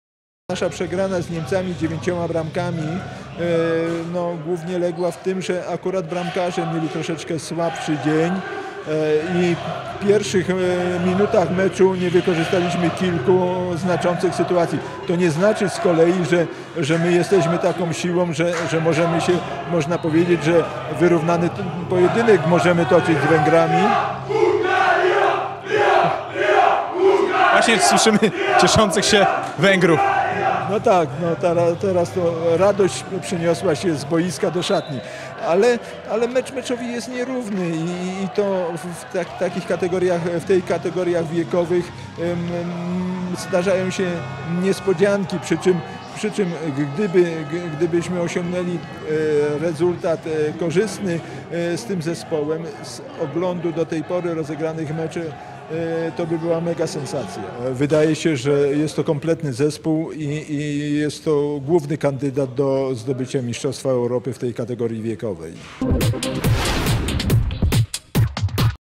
Wywiad z Zygfrydem Kuchtą, 20.08.2014
Zygfryd Kuchta o meczach reprezentacji Polski na Mistrzostwach Europy 2014 w piłce ręcznej mężczyzn do lat 18.